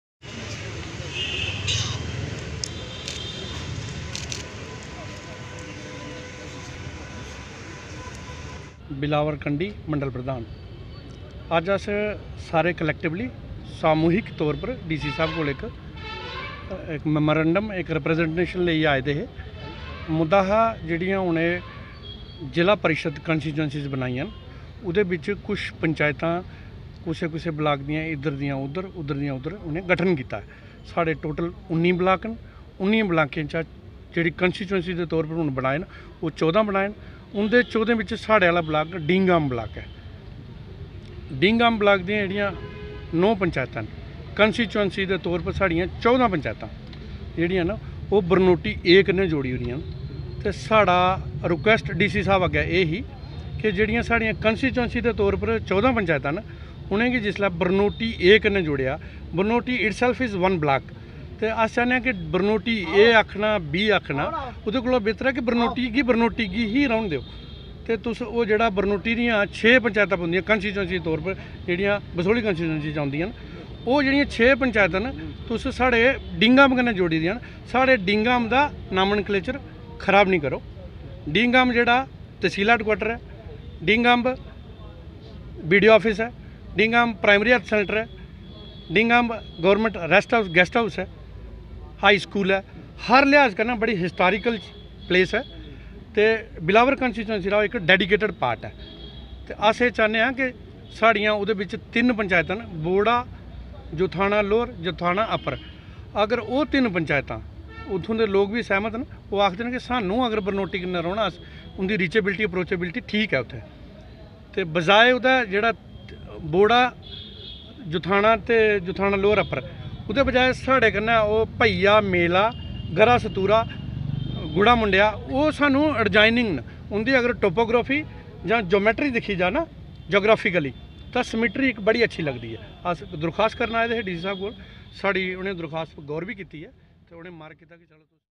The delegation meet DC Om Prakash Bhagat
The delegation meet DC Om Prakash Bhagat seeking to complete the process of  DDC panchayats properly